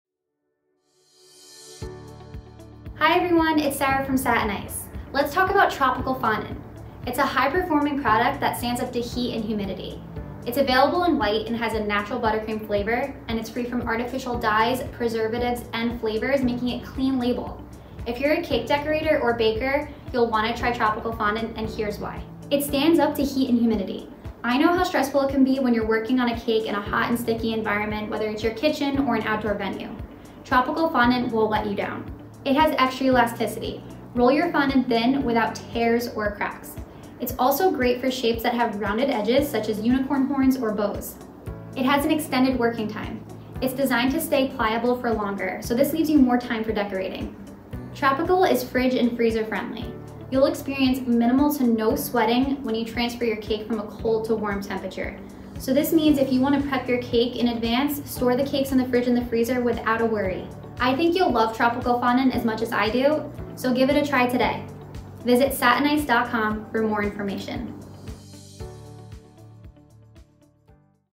Working with Satin Ice Tropical Formula Fondant | Features & Benefits
Sarah from Satin Ice showcases Tropical Formula Fondant!
• formulated for optimum performance in hot and humid conditions
• extended working & drying time
• extra elasticity
• no artificial colors, flavors or preservatives